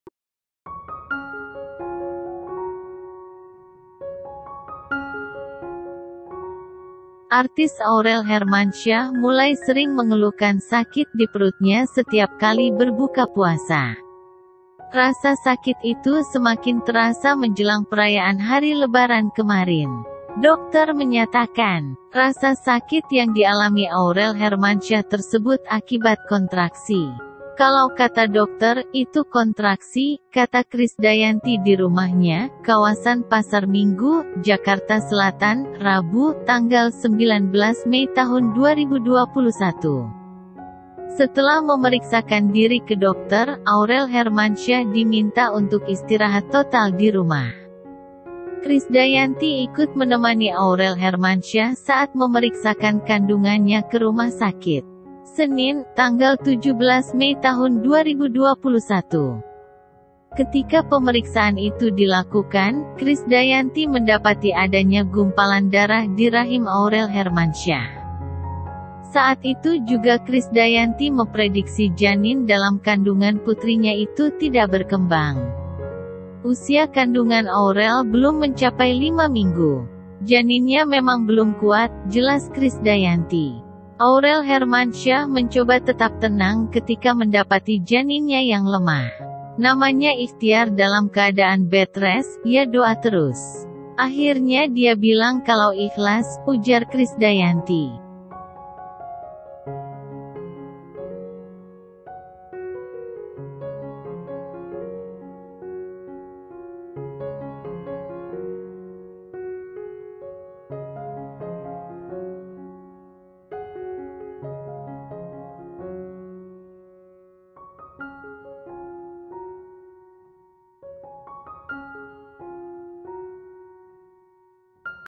Artis Krisdayanti Sebut Aurel Hermansyah Ikhlas Kehilangan Janin Bayi
TRIBUNLAMPUNG.CO.ID – Artis Aurel Hermansyah mulai sering mengeluhkan sakit di perutnya setiap kali berbuka puasa.
Rasa sakit itu semakin terasa menjelang perayaan Hari Lebaran kemarin.
Dokter menyatakan, rasa sakit yang dialami Aurel Hermansyah tersebut akibat kontraksi.
"Kalau kata dokter, itu kontraksi," kata Krisdayanti di rumahnya, kawasan Pasar Minggu, Jakarta Selatan, Rabu (19/5/2021).
Setelah memeriksakan diri ke dokter, Aurel Hermansyah diminta untuk istirahat total di rumah.
"Setiap kali setelah berbuka puasa, perut Aurel biasanya terasa sakit di malam hari," ujar Krisdayanti, ibu kandung Aurel Hermansyah.
Krisdayanti ikut menemani Aurel Hermansyah saat memeriksakan kandungannya ke rumah sakit, Senin (17/5/2021).
Ketika pemeriksaan itu dilakukan, Krisdayanti mendapati adanya gumpalan darah di rahim Aurel Hermansyah.
Saat itu juga Krisdayanti memprediksi janin dalam kandungan putrinya itu tidak berkembang.
Artikel ini telah tayang di WartaKotalive.com